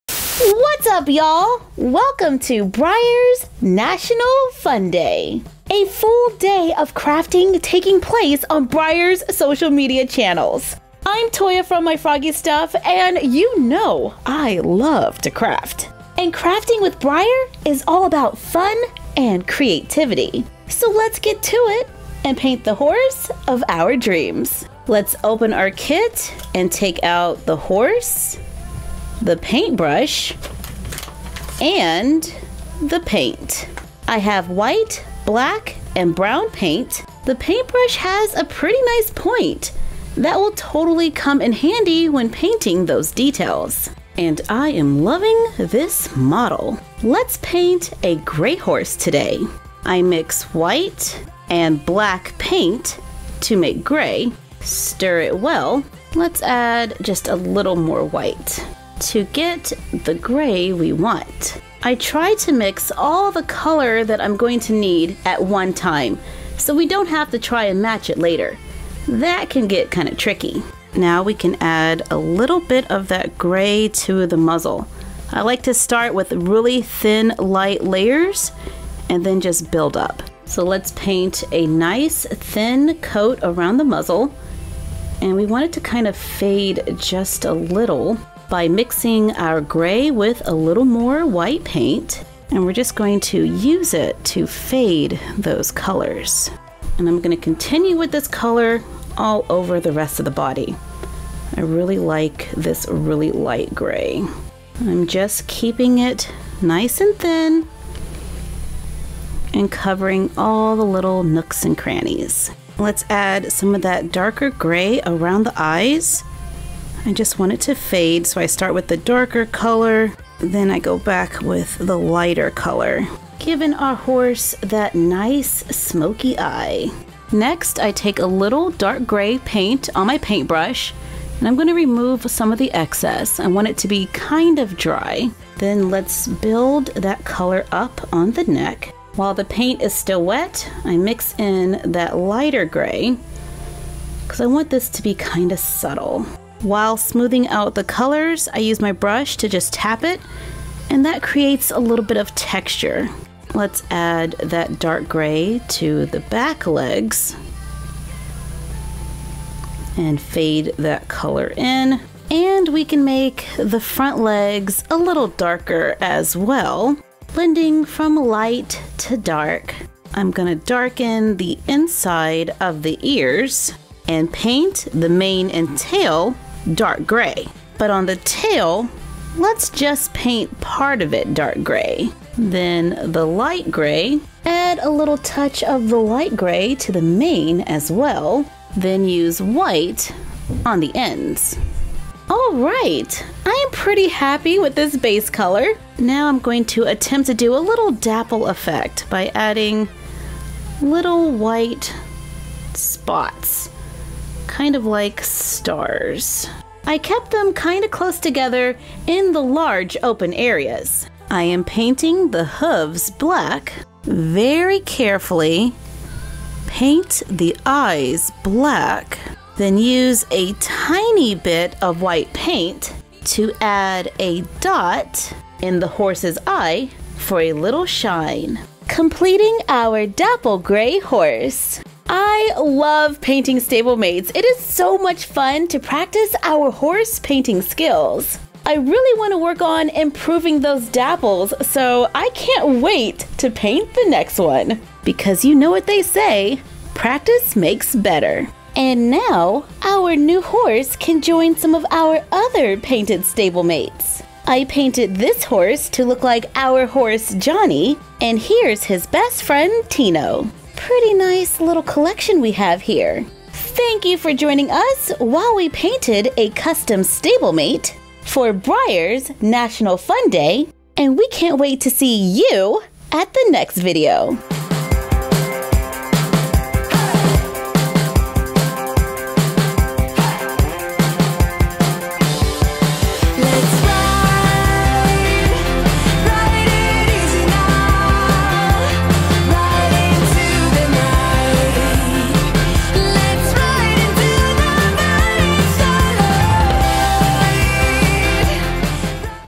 Painting a Grey Horse with @MyFroggyStuff | Breyer Model Horses' Virtual Fun Day
Our friend Toya of @MyFroggyStuff teaches you how to paint your very own dapple grey horse using one of Breyer's Horse Crazy Surprise painting kits! DIY the horse of your dreams with this fun tutorial created as part of the BreyerVirtualFunDay, held on October 17th, 2020 in celebration of Breyer's 70th anniversary. The simple techniques demonstrated in this video are perfect for beginner painters!